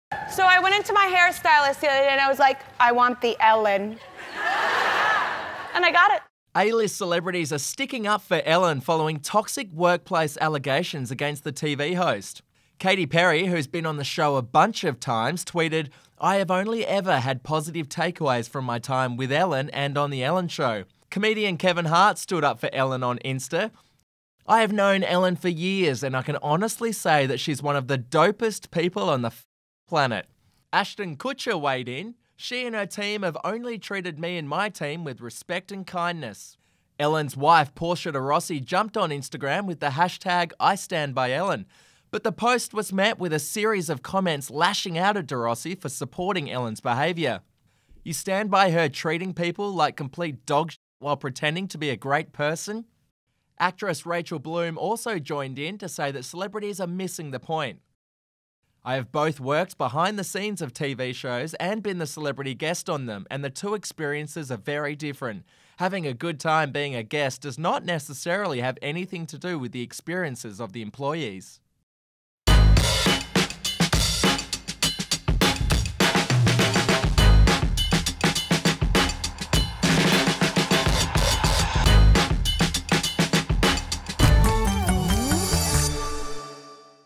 Ellen’s celeb friends come to her rescue
Katy Perry, Ashton Kutcher and Kevin Hart have come forward to support Ellen DeGeneres amid toxic workplace claims.